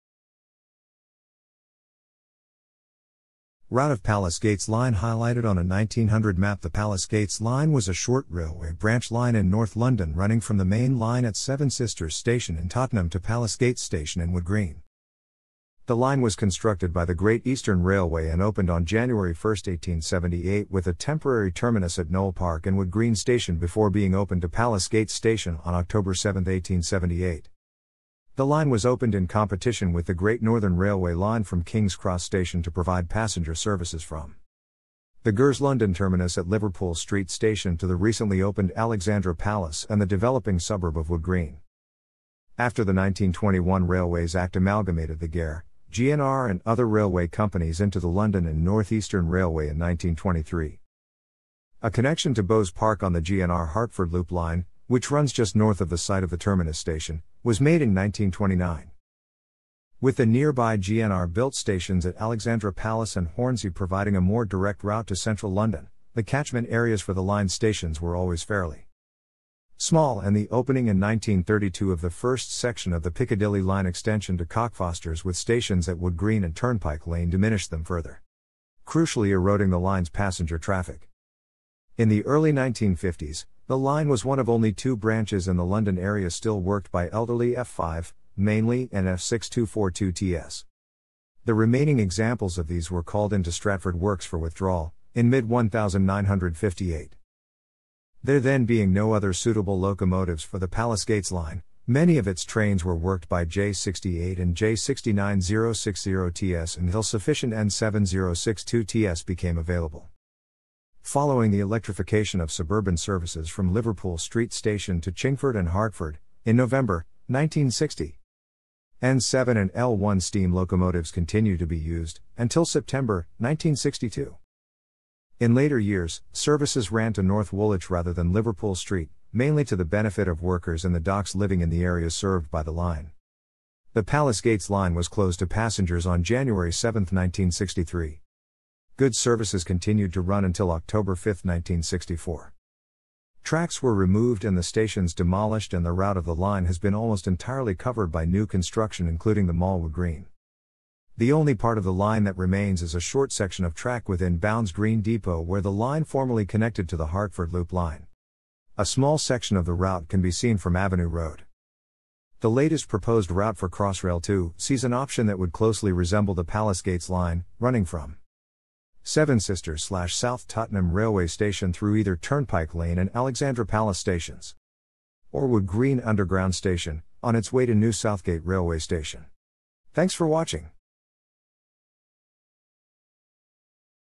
Palace Gates Line
Route of Palace Gates Line highlighted on a 1900 map The Palace Gates Line was a short railway branch line in north London running from the main line at Seven Sisters station in Tottenham to Palace Gates （Wood Green） station in Wood Green.
The line was constructed by the Great Eastern Railway （GER） and opened on 1 January 1878 with a temporary terminus at Noel Park and Wood Green station before being opened to Palace Gates （Wood Green） station on 7 October 1878.
The line was opened in competition with the Great Northern Railway （GNR） line from King's Cross station to provide passenger services from
the GER's London terminus at Liverpool Street station to the recently opened Alexandra Palace and the developing suburb of Wood Green.
After the 1921 Railways Act amalgamated the GER, GNR and other railway companies into the London and North Eastern Railway （LNER） in 1923,
a connection to Bowes Park on the GNR Hertford Loop Line, which runs just north of the site of the terminus station, was made in 1929.
With the nearby GNR built stations at Alexandra Palace and Hornsey providing a more direct route to central London, the catchment areas for the line's stations were always fairly
small and the opening in 1932 of the first section of the Piccadilly line extension to Cockfosters with stations at Wood Green and Turnpike Lane diminished them further,
crucially eroding the line's passenger traffic.
In the early 1950s, the line was one of only two branches in the London area still worked by elderly F5, mainly, and F6 2-4-2Ts.
The remaining examples of these were called into Stratford Works for withdrawal, in mid-1958.
There then being no other suitable locomotives for the Palace Gates line, many of its trains were wor...